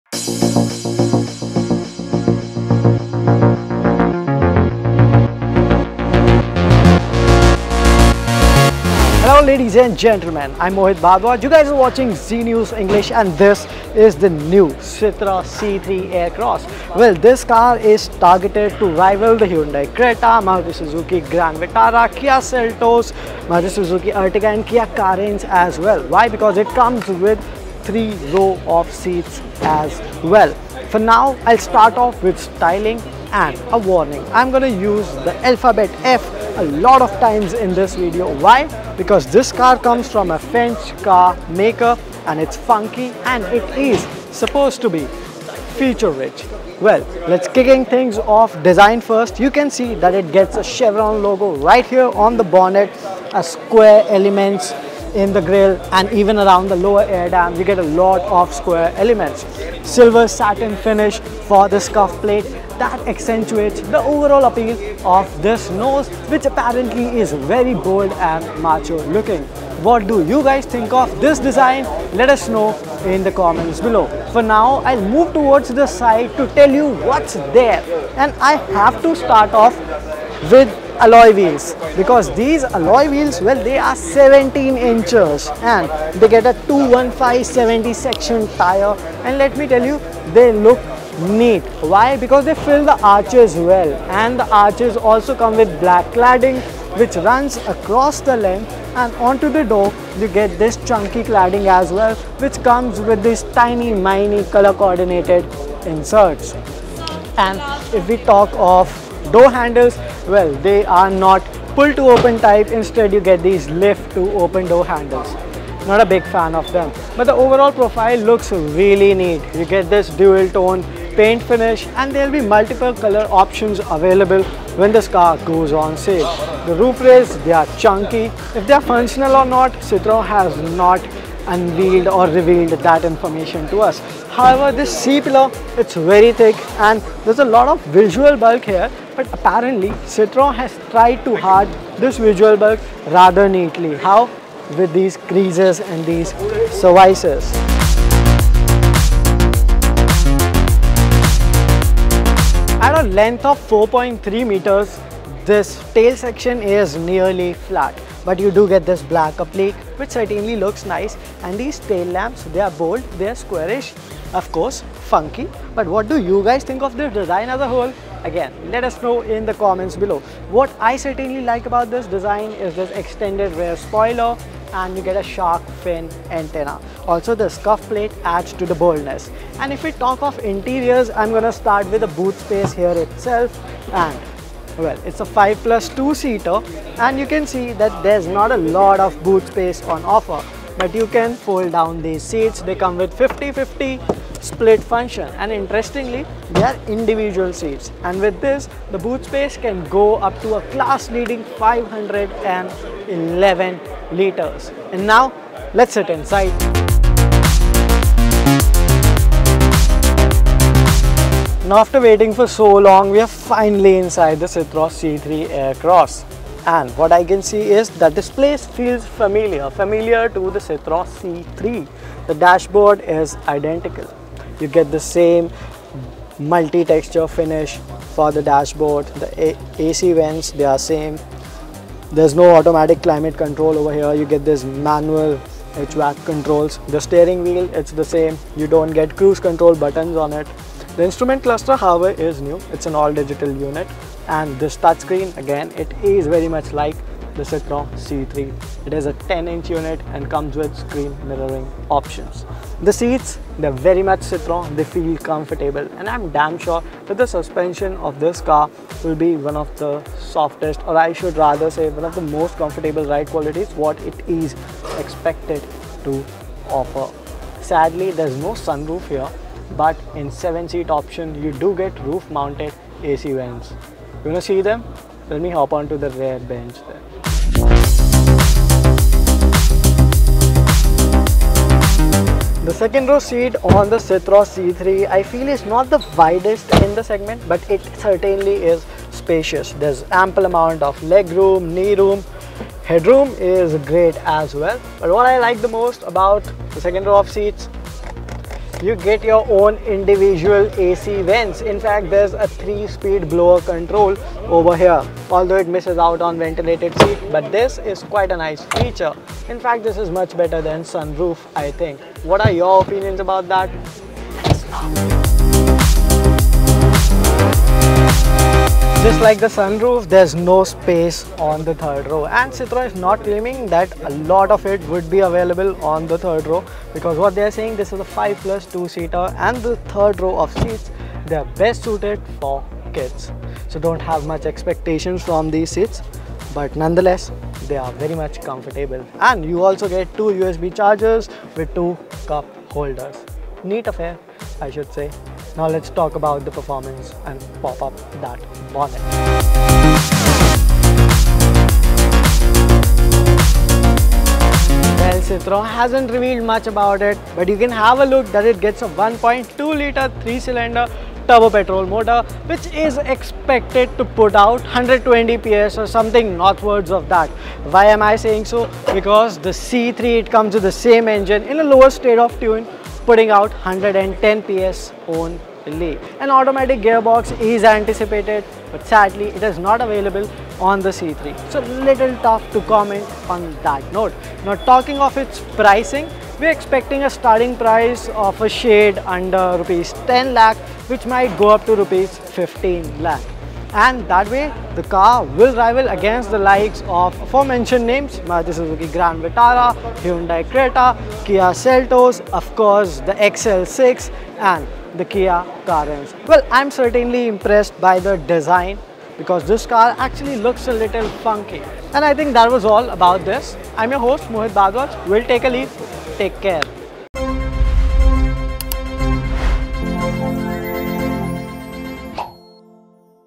2023 Citroen C3 Aircross First Look: Perfect Mid SUV For India? | Zee News English
2023 Citroen C3 Aircross First Look: Perfect Mid SUV For India? | Zee News English